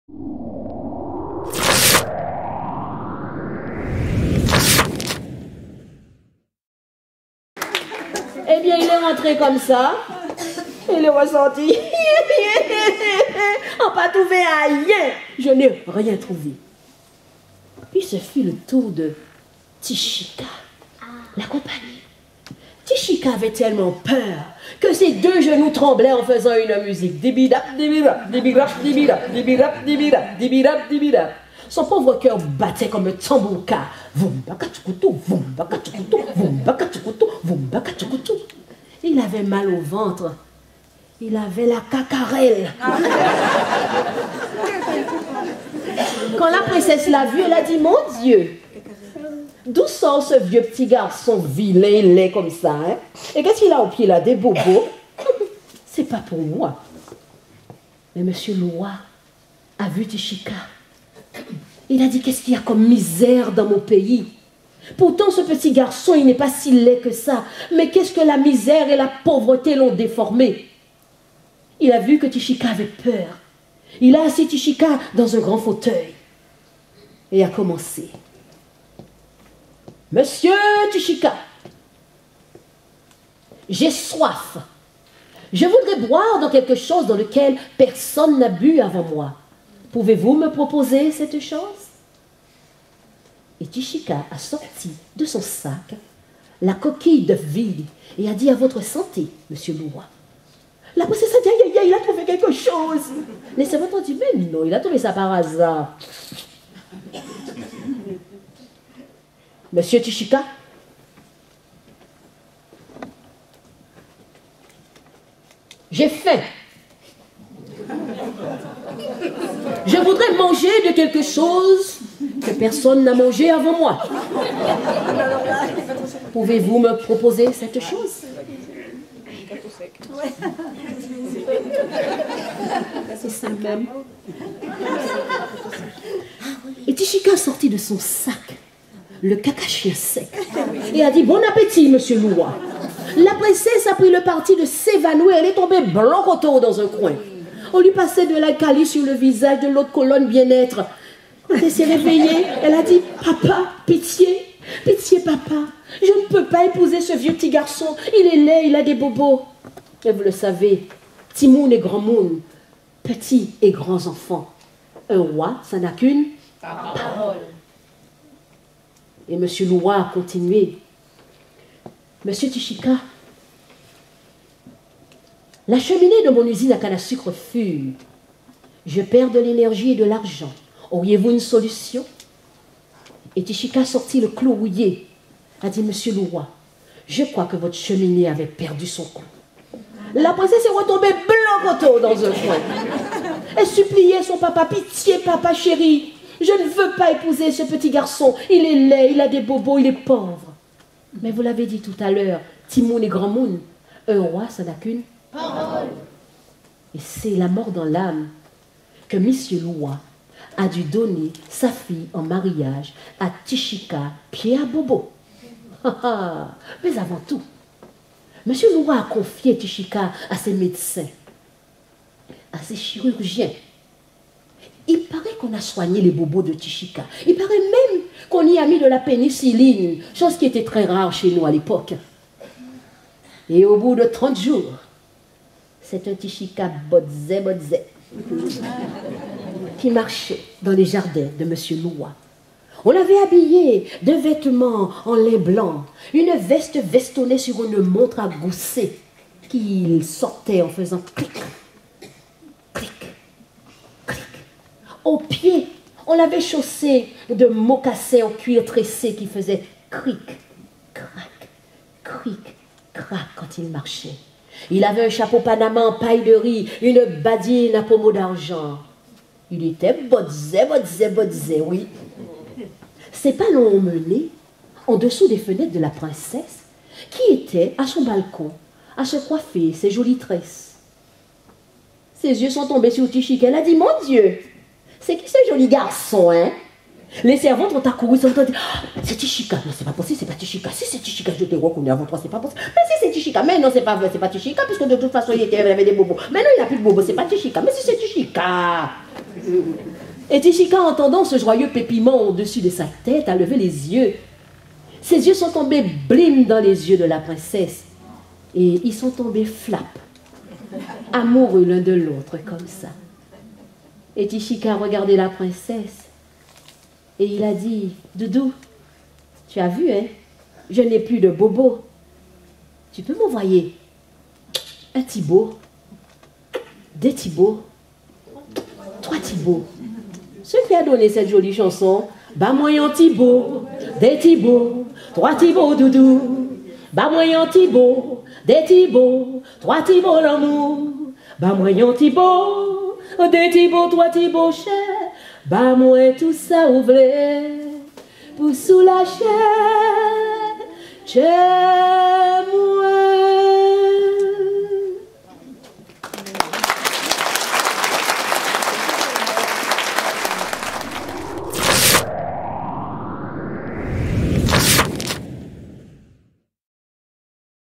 Magguy Faraux : Contes et chants de ma case créole à Villebrumier.
.… Et la princesse épousa Ti Chika …